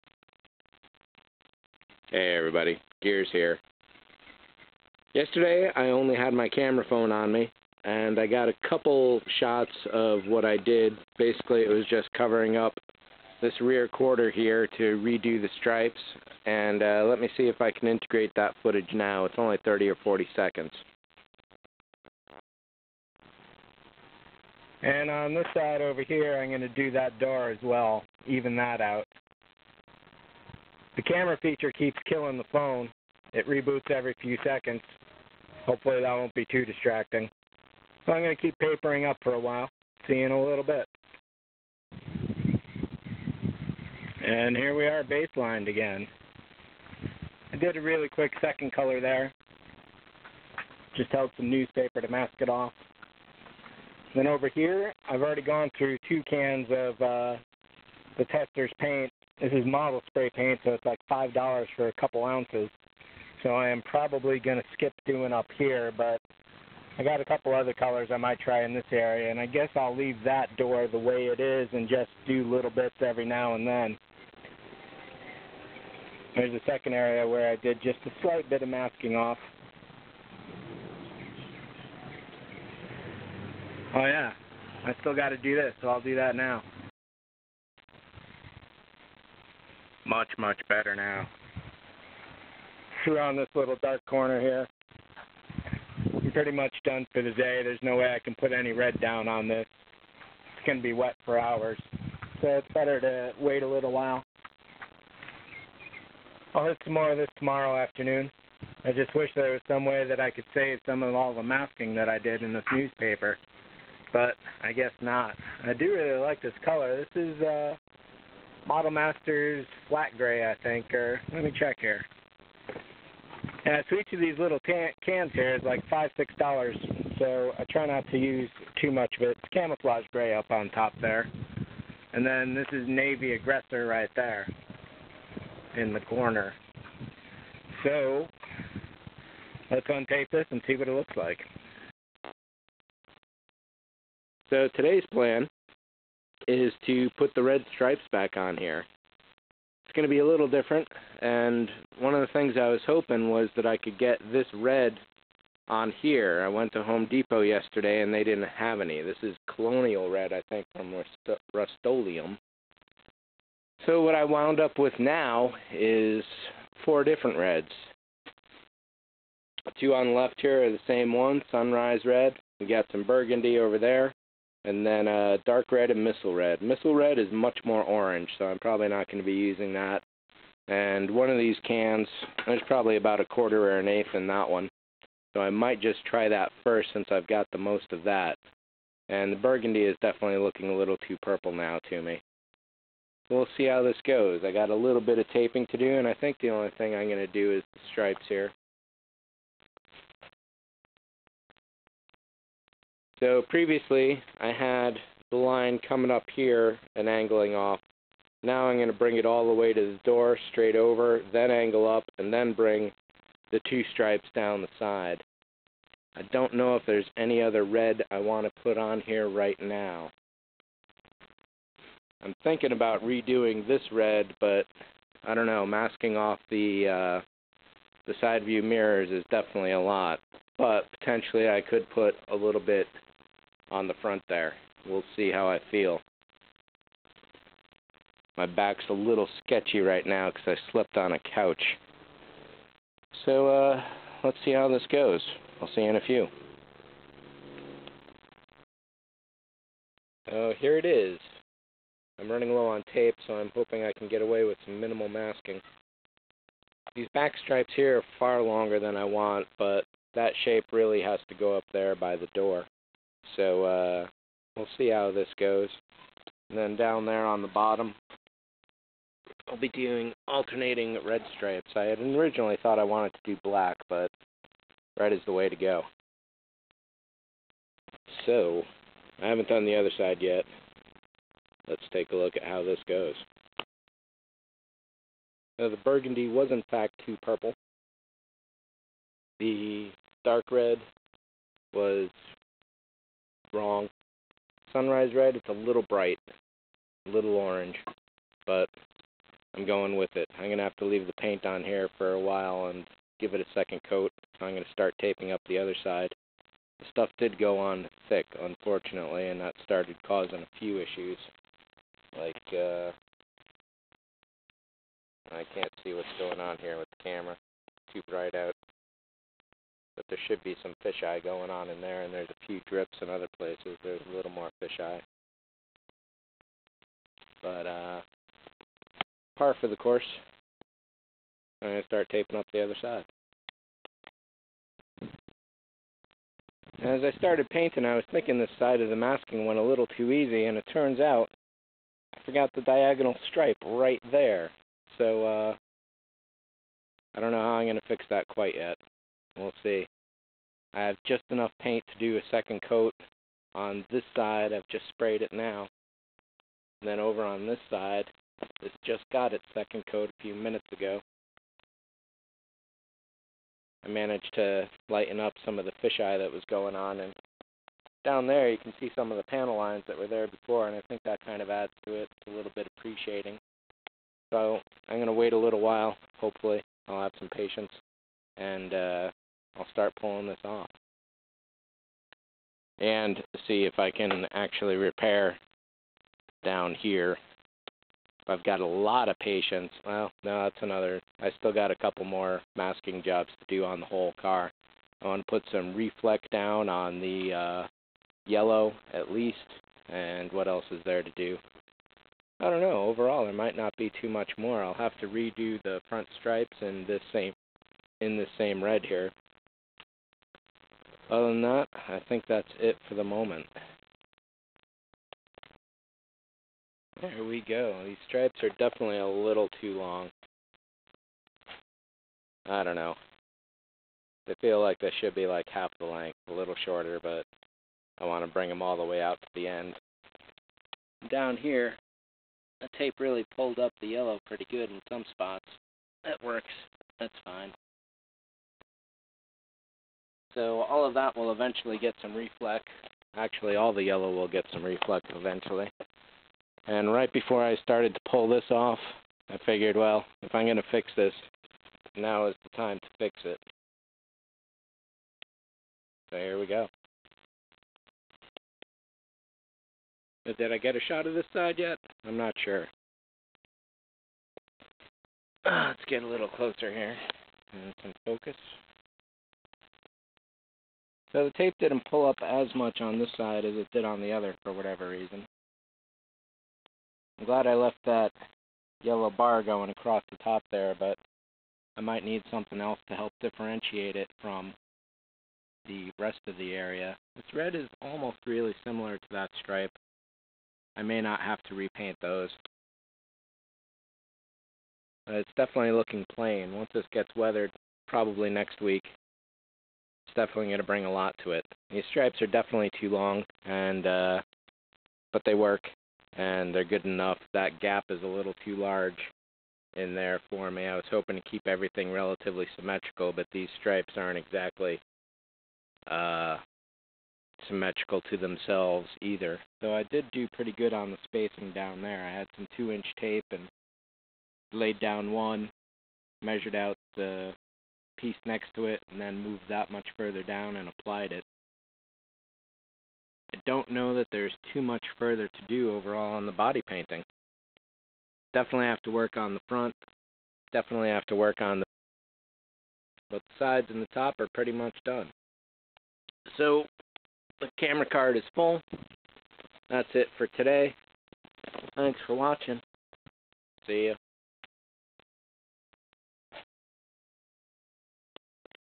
Project CarFighter: 10 Now we're talking!
Working from a vague concept and winging the actual design, I'm painting my car star wars style.

It started a few years ago when I scraped up the psgr side door on a pylon.  With the the car paid off, I figure it's time to have fun!

This is my third such vehicle painted. A Suzuki Bergman and a '96 Chevy Corsica hatchback previously, now it's a Tiburon.